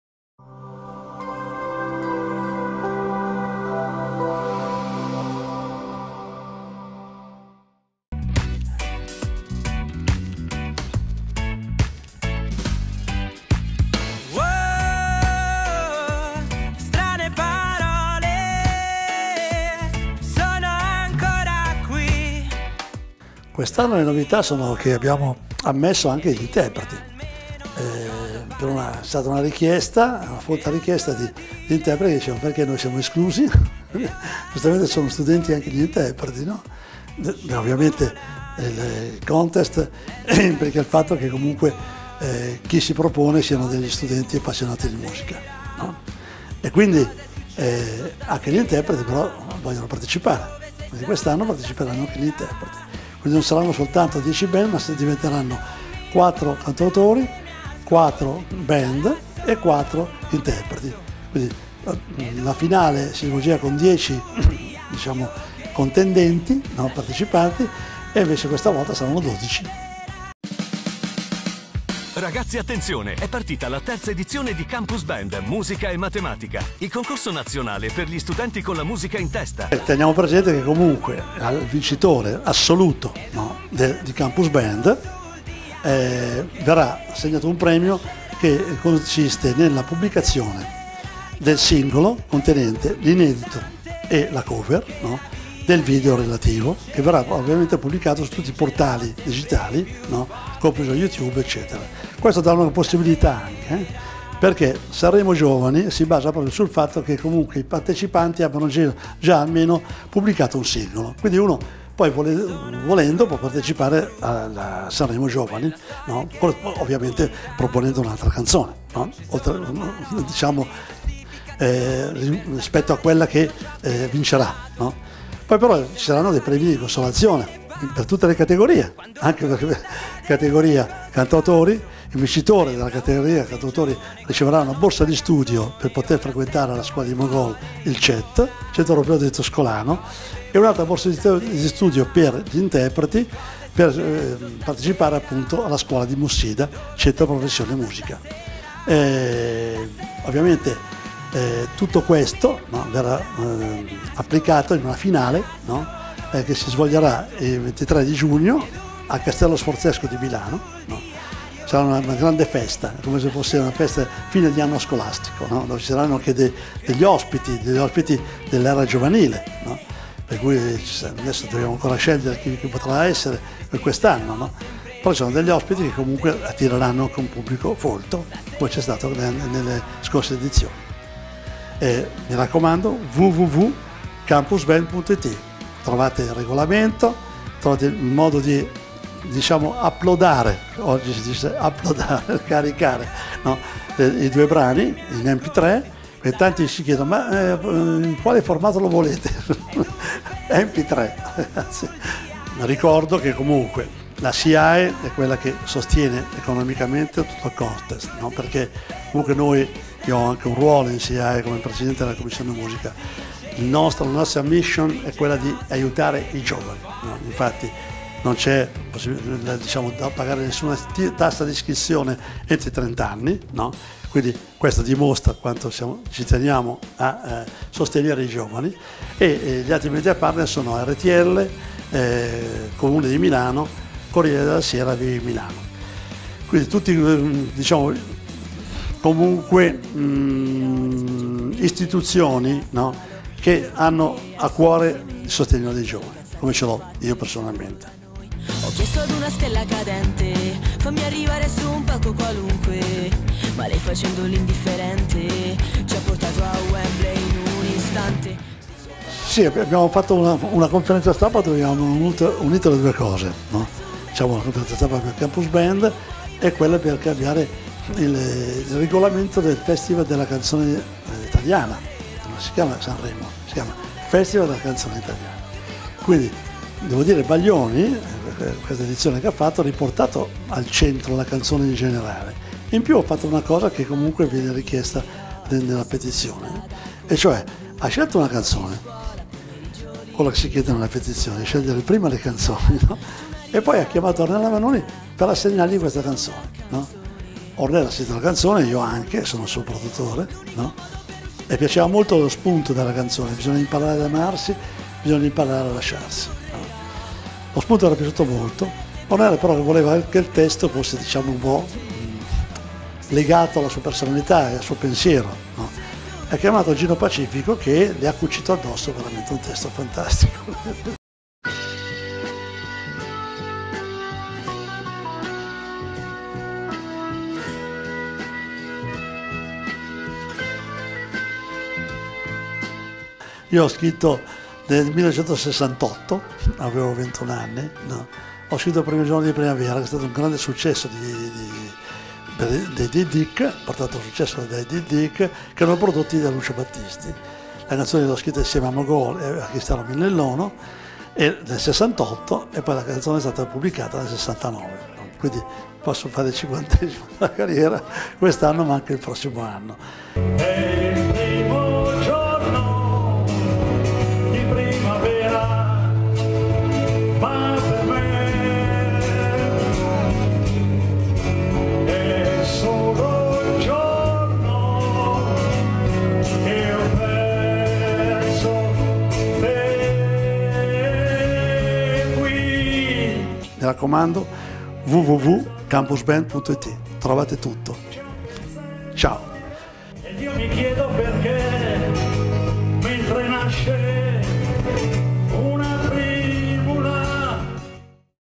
Mario Lavezzi presenta la terza edizione di Campusband
Autore, cantautore, produttore e scopritore di talenti ha raccontato ai nostri microfoni la nuova edizione del concorso nazionale dedicato agli studenti che, oltre ad impegnarsi nello studio, hanno anche la passione per le sette note